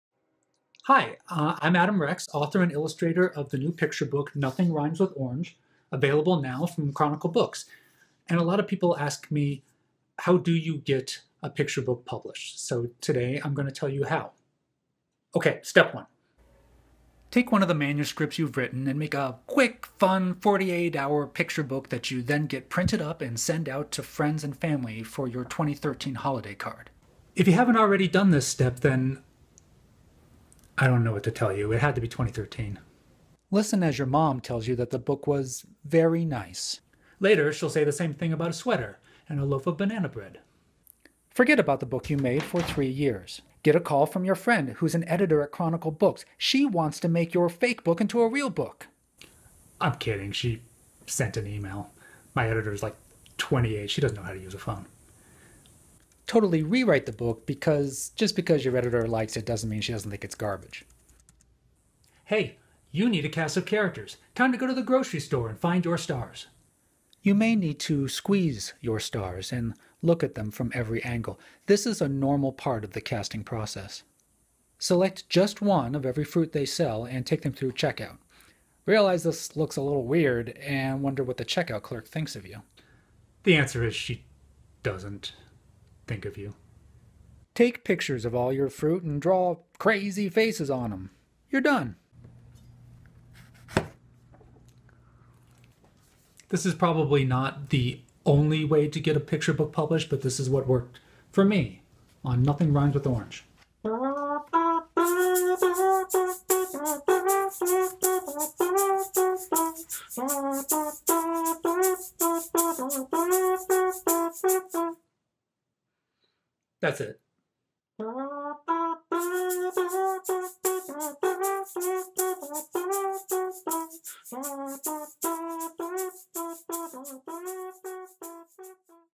How To Get Your Picture Book Published- Advice From Adam Rex
Adam Rex's celebrated picture book Nothing Rhymes With Orange came about in an unusual way. In this video, Adam explains his top tips for getting a picture book published with a delicious end.

About Nothing Rhymes With Orange
By Adam Rex
We all know nothing rhymes with orange. But how does that make Orange feel? Well, left out! When a parade of fruit gets together to sing a song about how wonderful they are—and the song happens to rhyme—Orange can't help but feel like it's impossible for him to ever fit in. But when one particularly intuitive Apple notices how Orange is feeling, the entire English language begins to become a bit more inclusive. Beloved author-illustrator Adam Rex has created a hilarious yet poignant parable about feeling left out, celebrating difference, and the irrefutable fact that nothing rhymes with orange.